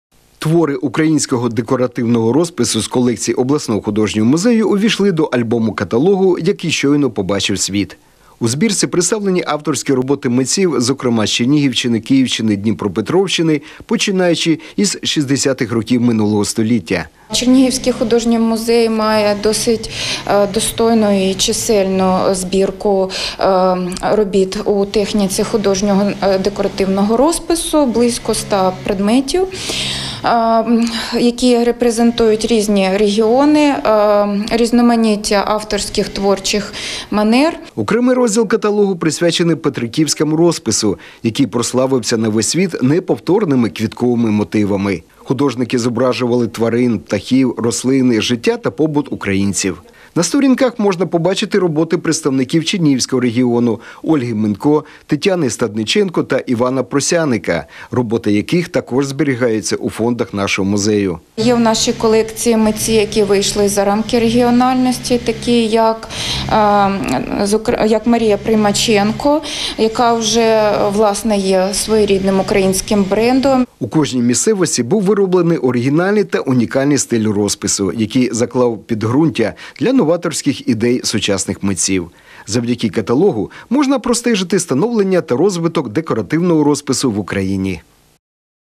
Художній музей видав новий альбом-каталог
Твори українського декоративного розпису з колекції обласного художнього музею увійшли до альбому-каталогу, який щойно побачив світ. У збірці представлені авторські роботи митців, зокрема з Чернігівщини, Київщини, Дніпропетровщини, починаючи з 60-х років минулого століття.
Окремий розділ каталогу присвячений Петриківському розпису, який прославився на весь світ неповторними квітковими мотивами. Художники зображували тварин, птахів, рослини, життя та побут українців. На сторінках можна побачити роботи представників Чернігівського регіону - Ольги Минко, Тетяни Стадниченко та Івана Просяника, роботи яких також зберігаються у фондах нашого музею.
У кожній місцевості був вироблений оригінальний та унікальний стиль розпису, який заклав підґрунтя для новаторських ідей сучасних митців. Завдяки каталогу можна простежити становлення та розвиток декоративного розпису в Україні.